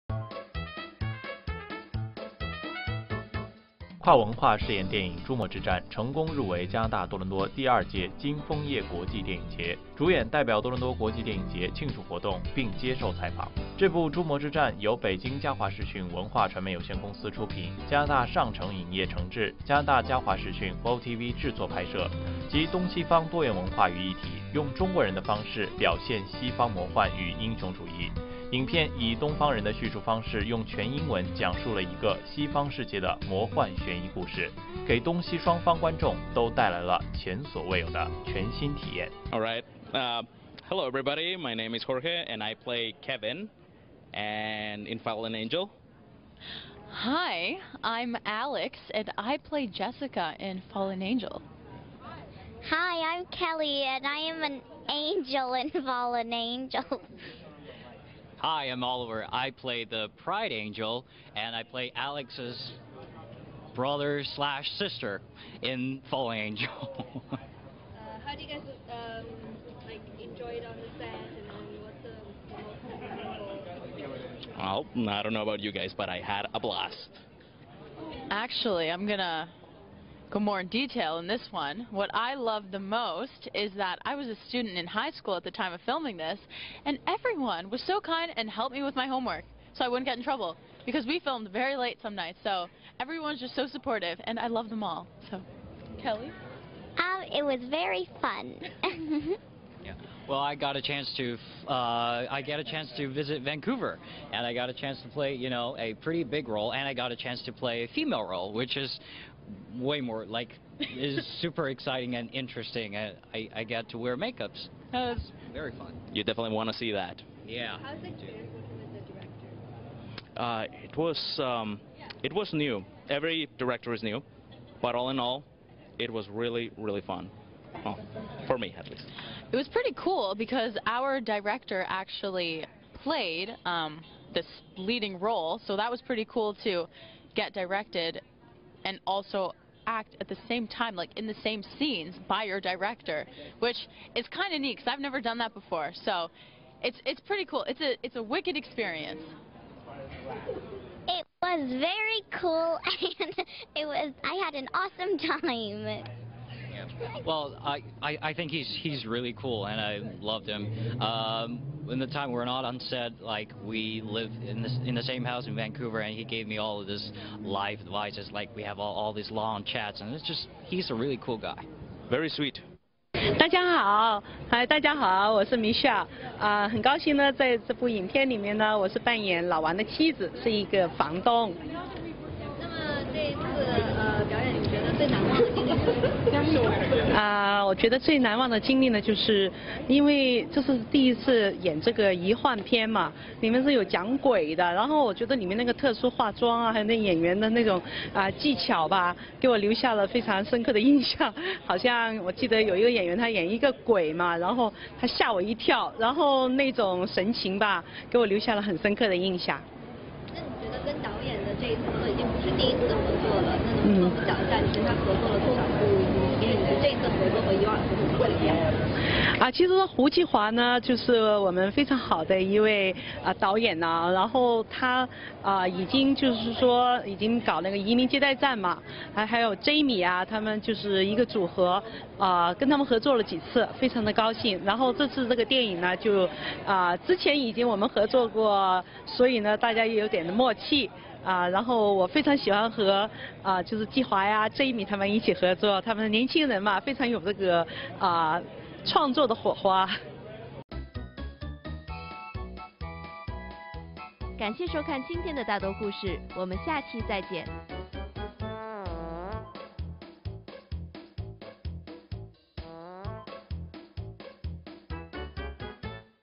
0918 《諸魔之戰》成功入圍加拿大多倫多第二屆金楓葉國際電影節
0918 《諸魔之戰》成功入圍加拿大多倫多第二屆金楓葉國際電影節
Life@5